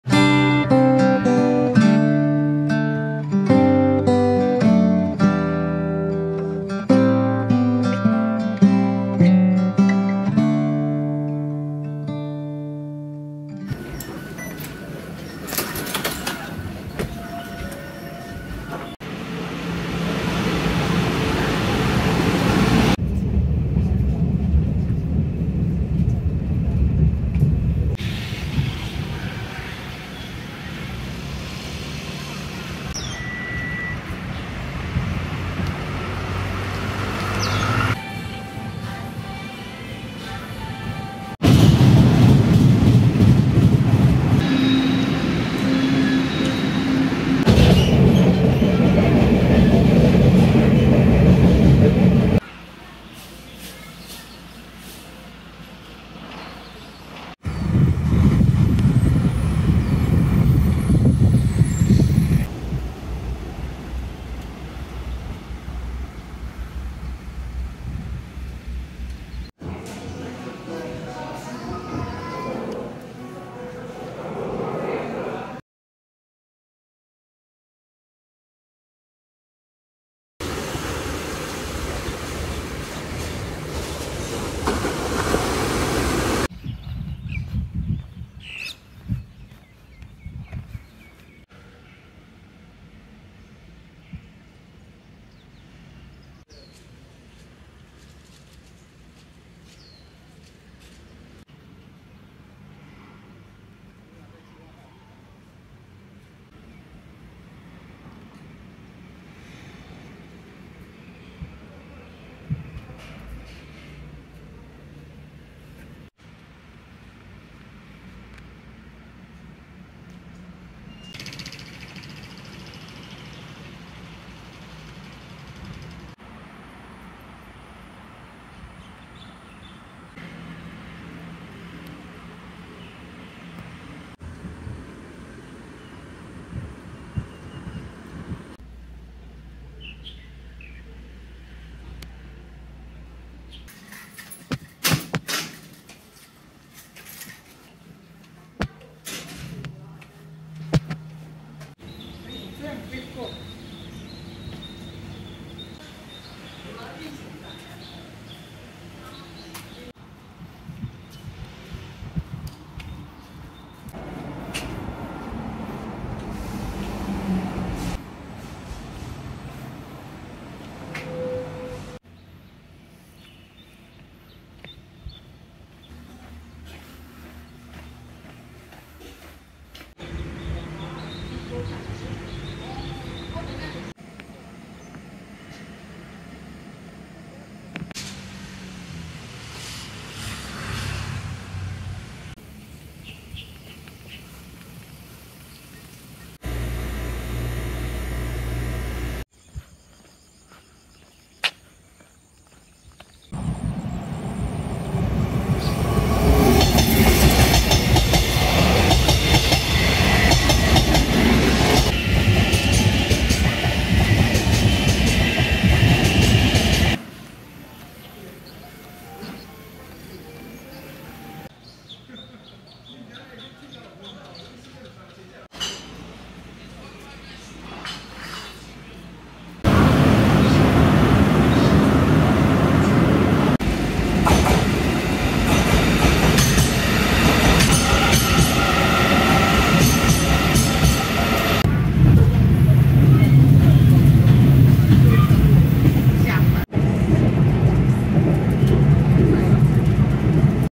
[vlog] 宜蘭頭城一日遊 | 頭城老街散步、滿山望海看太平洋、頭圍步道追火車
📍頭城老街
📍滿山望海咖啡
📍頭城文創園區
📍頭圍景觀步道